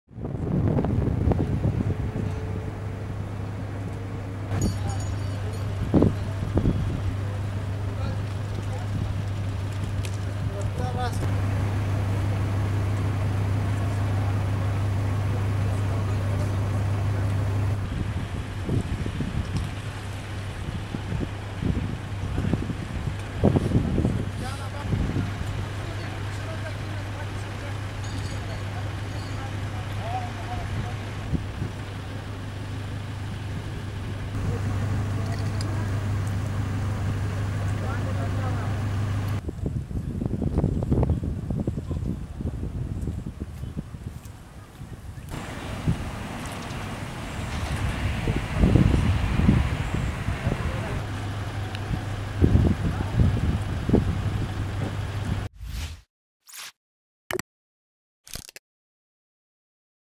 Afghan official killed by car bomb in Kandahar
Images of the scene after Kandahar governor's secretary was killed when a bomb attached to his car blew up near their office compound in the southern Afghan city. The blast comes as the Taliban captured a key district in the province, their former bastion, after fierce night-time fighting with Afghan government forces.